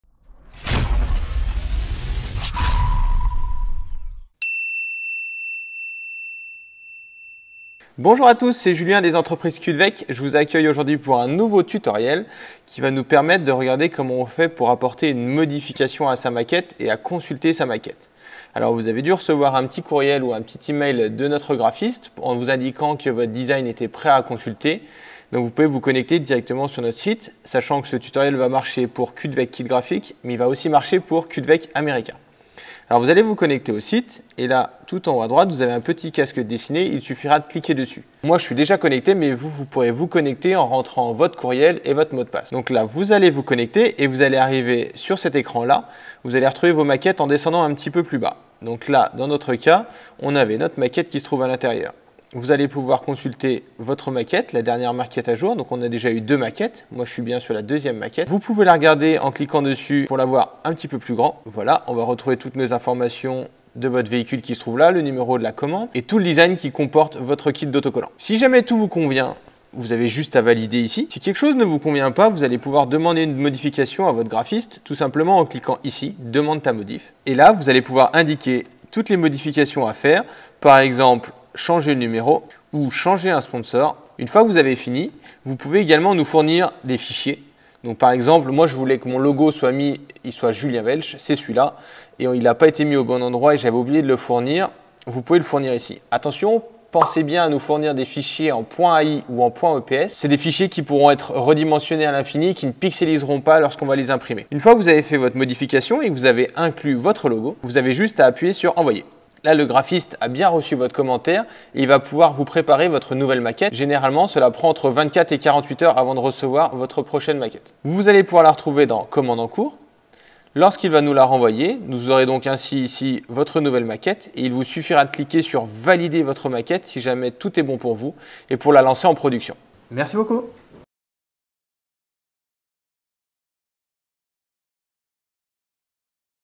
Consulte et Modifie ta commande - Kutvek Kit Graphique
Vous souhaitez apporter une modification à un design, voici un tutoriel qui va vous aider à indiquer cela au graphiste en charge de votre projet.

Plus de kit graphique motocross, quad, jet ski, moto neige sur